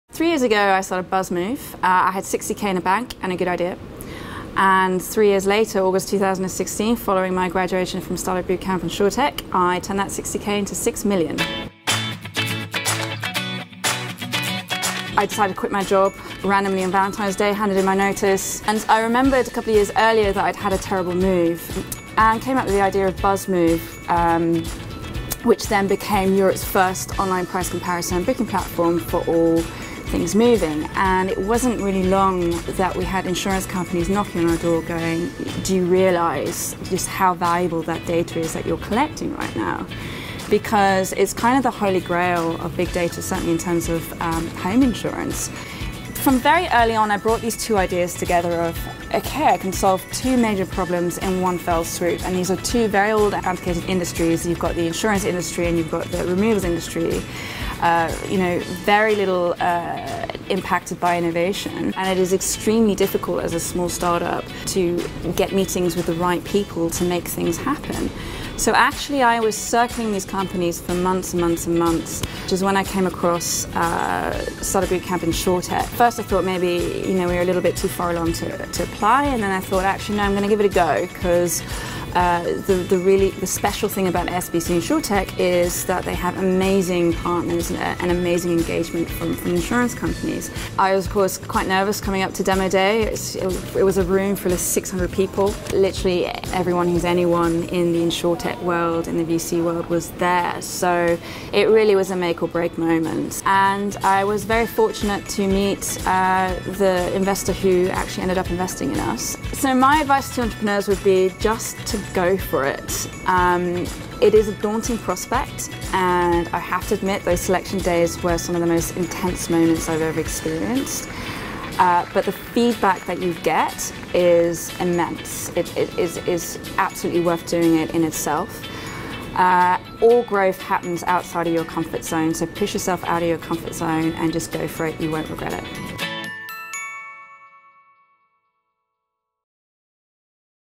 Startupbootcamp InsurTech Alumni: Buzzmove's Journey to £6 Million Super-Seed Round
Buzzmove graduated Startupbootcamp InsurTech's accelerator program in April 2016. In August 2016, Buzzmove secured a £6 Million ($8 Million) super-seed round investment. This is their story.